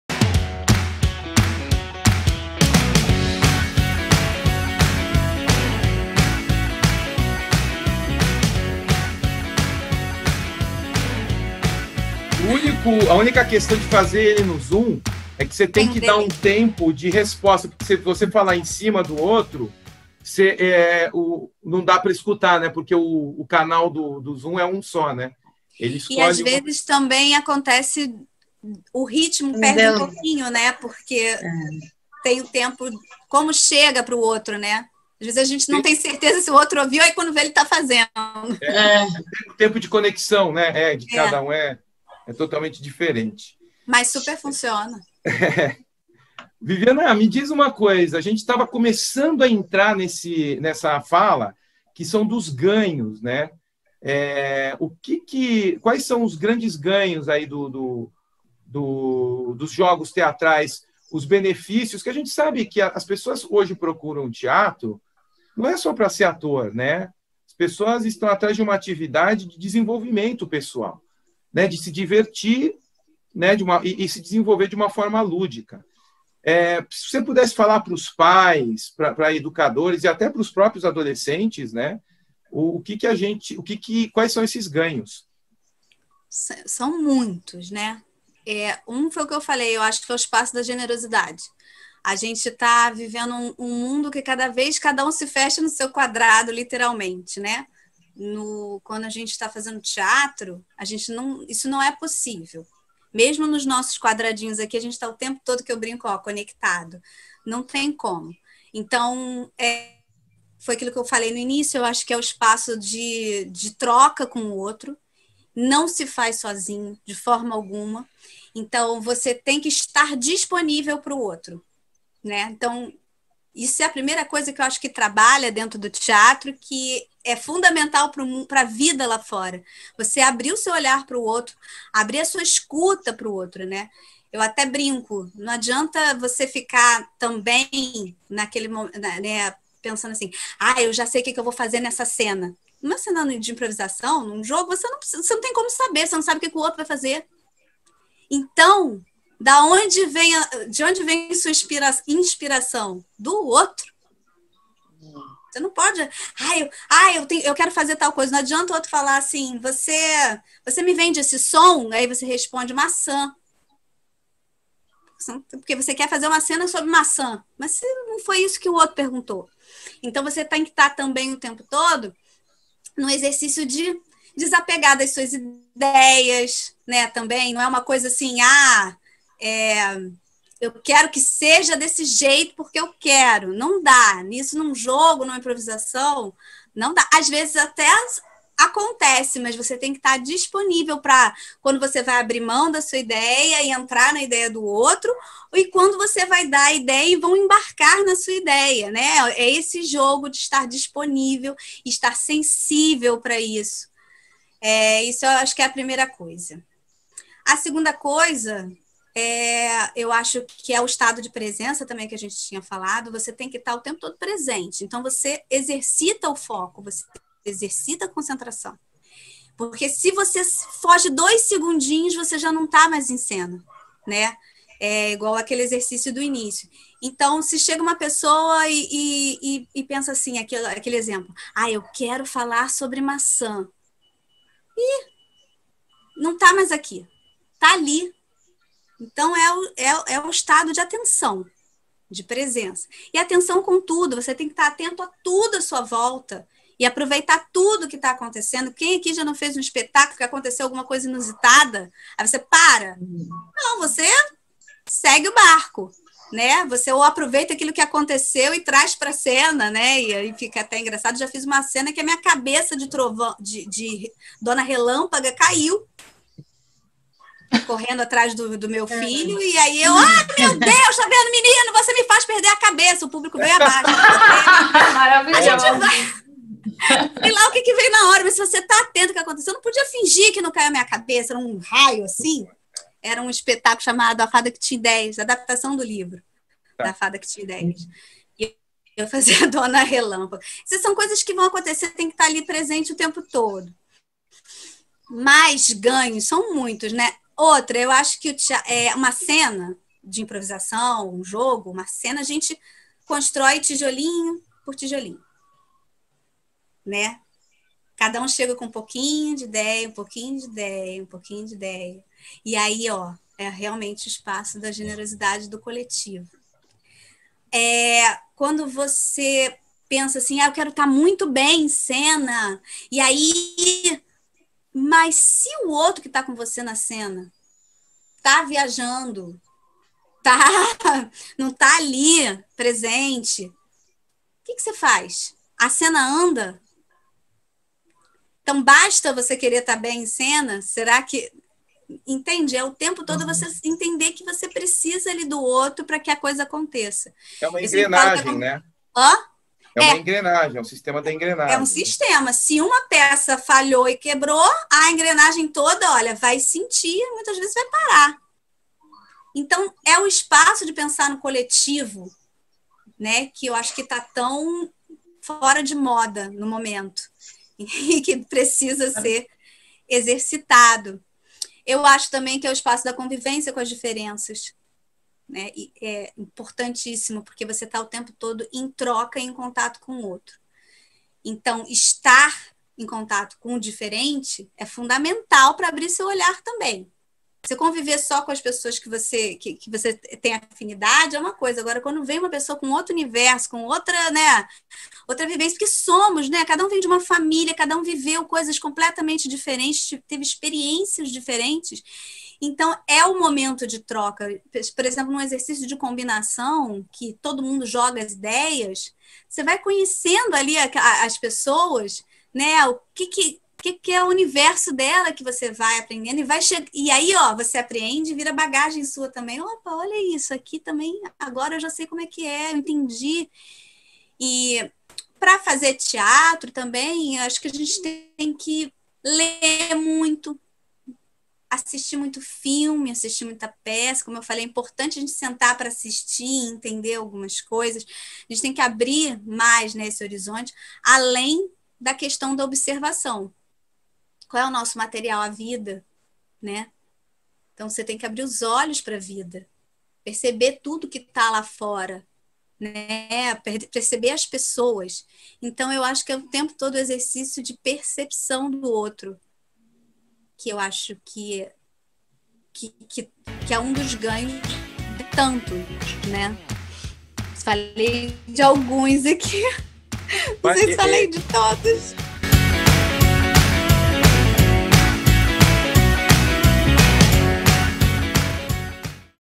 BENEFÍCIOS DAS AULAS DE TEATRO - CORTES DO TEATRANDO com Viviana Rocha
São tantos! Aqui a Viviana faz uma reflexão sobre alguns deles. Se você acha que é só perder a timidez, está muito engando.

Quer tirar alguma dúvida ou quer saber mais sobre o Teatrando? Entra em contato!
A gente espera teu contato!